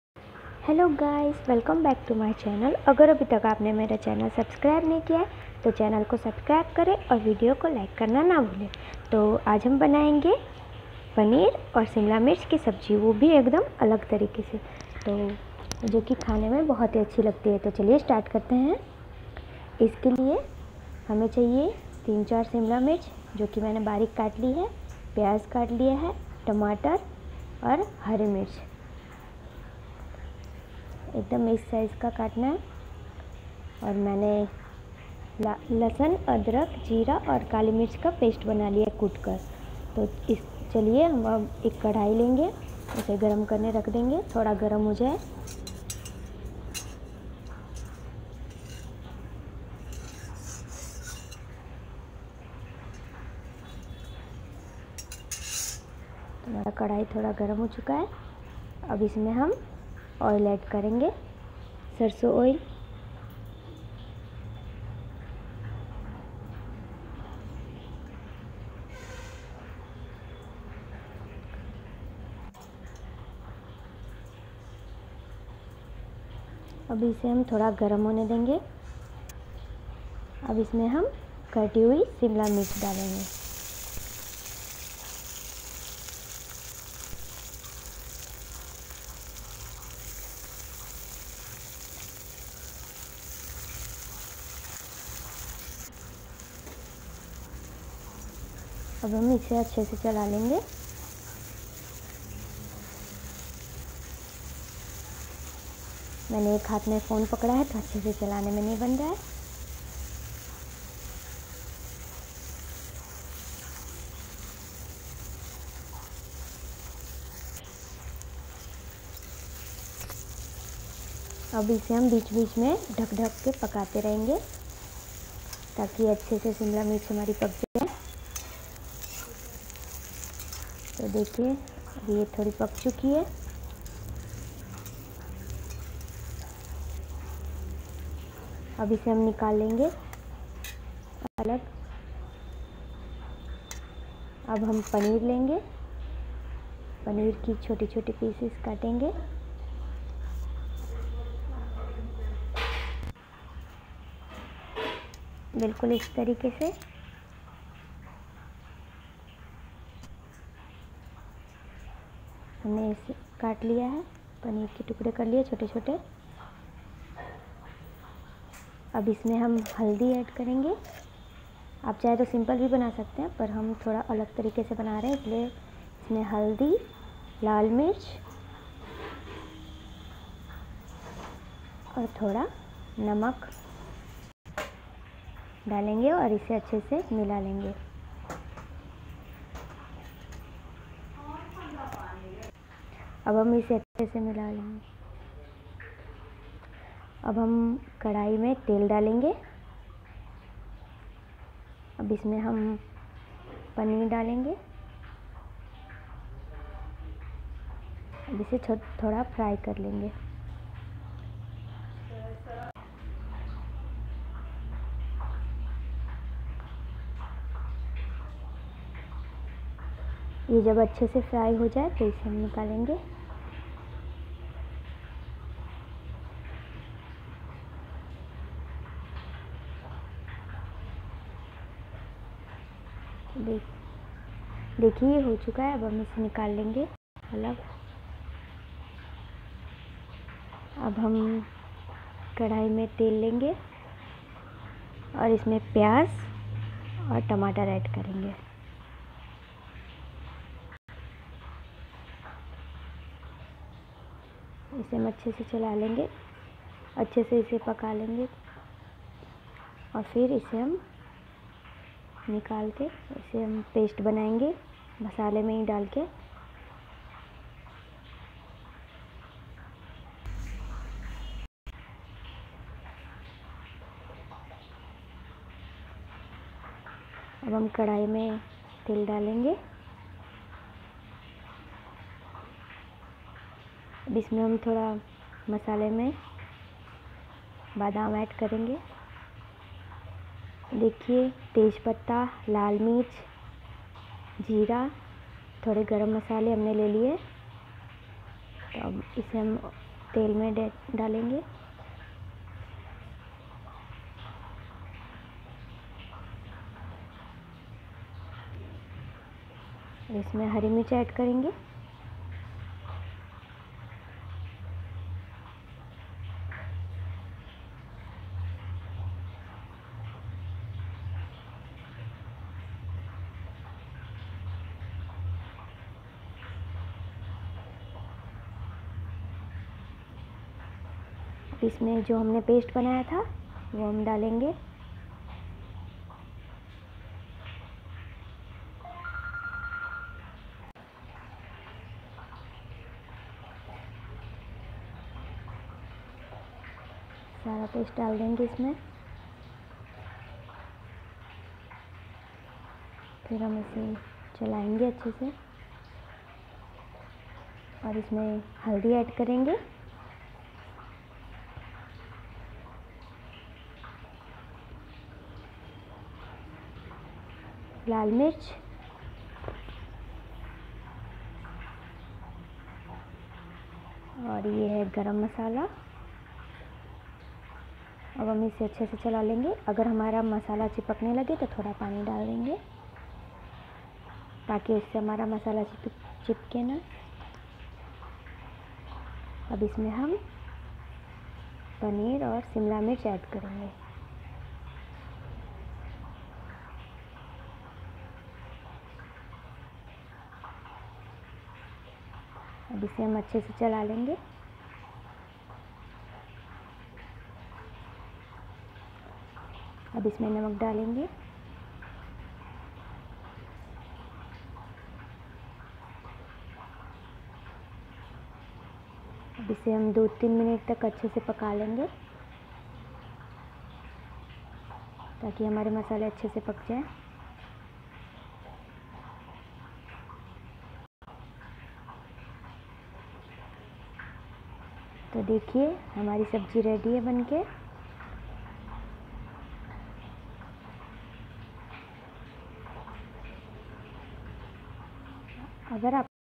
ऐसे बनाइए पनीर शिमला मिर्च की सब्जी एकदम अलग तरीके से बहुत ही easy तरीके से
हाय फ्रेंड्स कैसे हो आप सब आज मैंने इस वीडियो में आपको बताया है पनीर और शिमला मिर्च की सब्जी बनाना और यह बहुत ही आसानी से बन जाता है बहुत इसी तरीके से और यह खाने में भी बहुत स्वादिष्ट लगता है तो एक बार आप जरूर ट्राई करें और अगर आपको हमारा वीडियो पसंद आए तो वीडियो को लाइक करें और चैनल को सब्सक्राइब करो थैंक यू